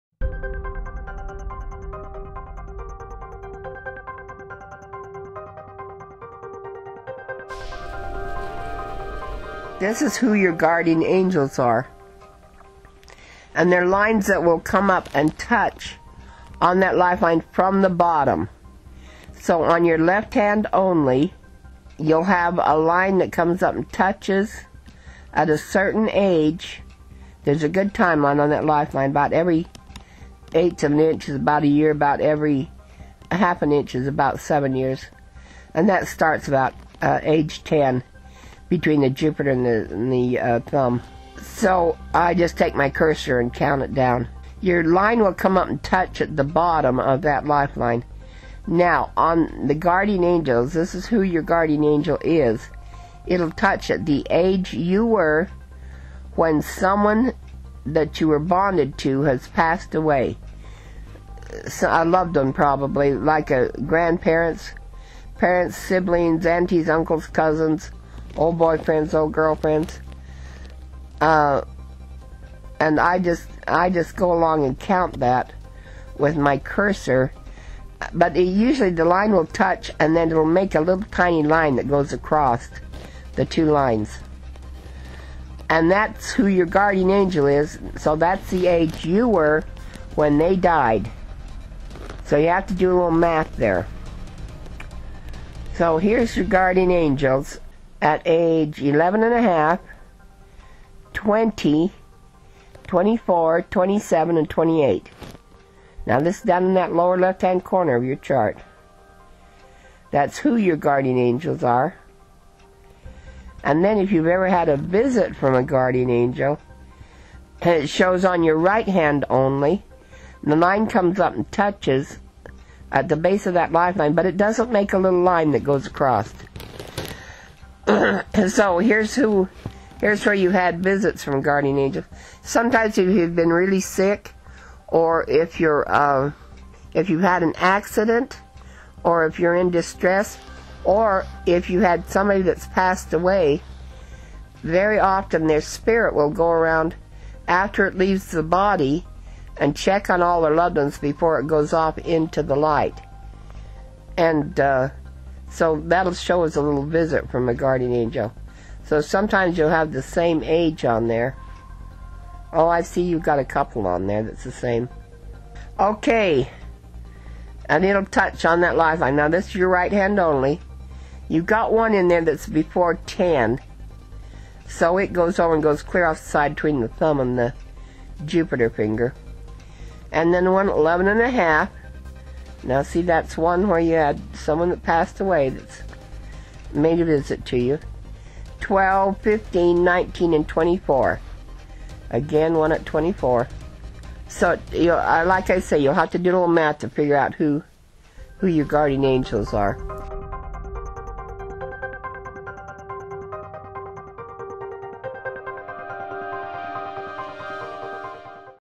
YOUR GUARDIAN ANGELS ARE REAL - WATCH THIS TO SEE WHY !
Your Left and Right hands will reveal two different realities about your Guardian Angels.

Kat Anders has a Master's Degree in the Health Sciences, a Bachelors Degree in music, and has performed over 6000 hand readings for well over 35 years.

Kat is developing 'Kat's Krew' of seasoned Analysts.